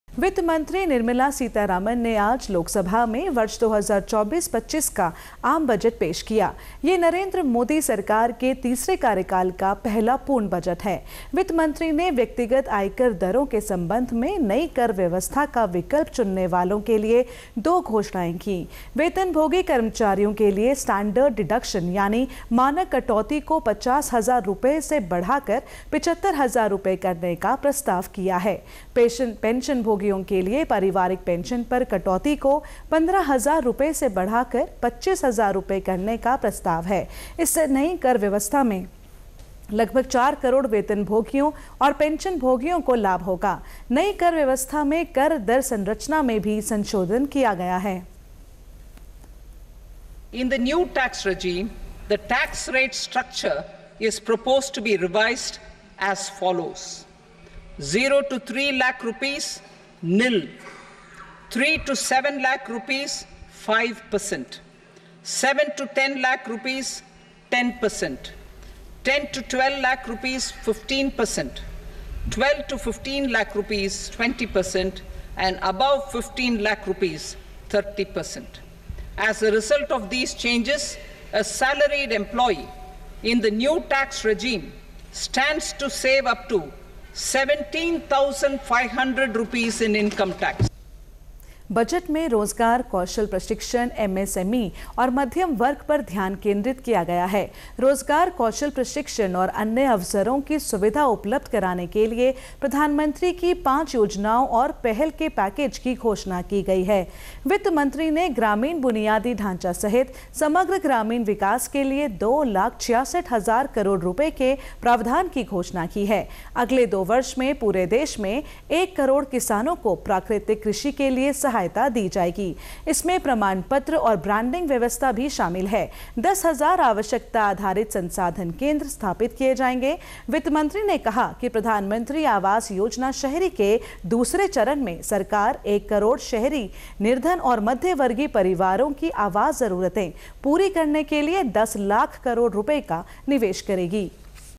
वित्त मंत्री ने आज लोकसभा में वर्ष 2024-25 का आम बजट पेश किया
वित्त मंत्री निर्मला सीतारामण ने आज लोकसभा में वर्ष 2024-25 का आम बजट पेश किया। यह नरेन्द्र  मोदी सरकार के तीसरे कार्यकाल का पहला पूर्ण बजट है। वित्तमंत्री ने व्यक्तिगत आयकर दरों के संबंध में नई कर व्यवस्था का विकल्प चुनने वालों के लिए दो घोषणाएं कीं।